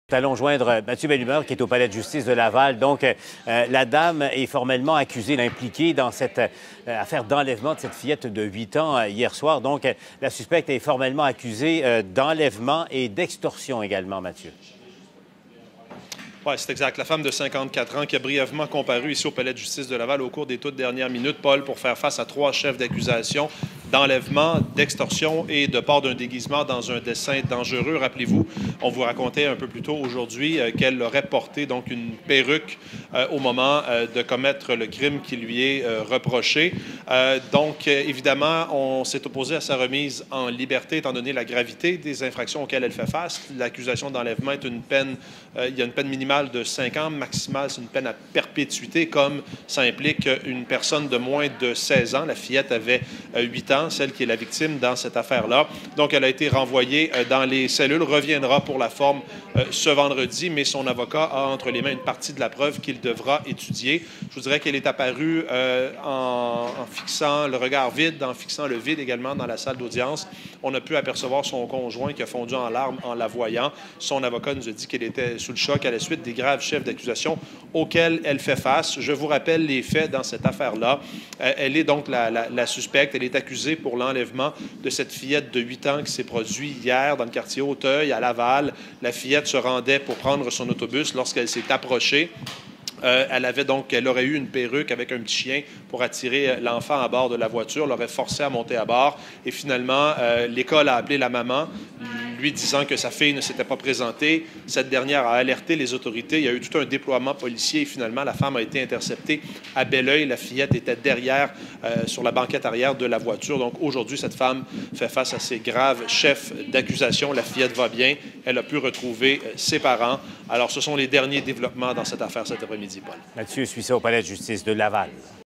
Enfant enlevée à Laval: une femme proche de la famille accusée
Une femme proche de la famille de l’enfant de 8 ans qui a été enlevée mardi à Laval a été accusée d'enlèvement, d’extorsion et de port d'un déguisement dans un dessein dangereux.

Une fillette de huit ans kidnappée et retrouvée ligotée

La femme de 54 ans a brièvement comparu mercredi au palais de justice de Laval. Elle est restée calme pendant la comparution et fixait le vide. Son conjoint a fondu en larmes en la voyant.

En raison de la gravité des trois chefs d’accusation, la Couronne s’est opposée à la remise en liberté et elle demeure détenue pour le moment.

L'accusée sera de retour en cour vendredi.

Rappel des faits
L’accusée s’est approchée de l’enfant alors que celle-ci se rendait à l’école. Il semblerait qu’elle portait une perruque et avait un petit chien pour attirer la fillette.

Elle aurait forcé la petite à monter à bord de la voiture et l’aurait mise sur la banquette arrière de son véhicule.

Voyant que l’enfant ne s’était pas présenté à l’école, l’établissement scolaire a avisé les parents. C’est là que les policiers ont entamé des recherches.

La femme a finalement été arrêtée par les autorités à Beloeil, en Montérégie.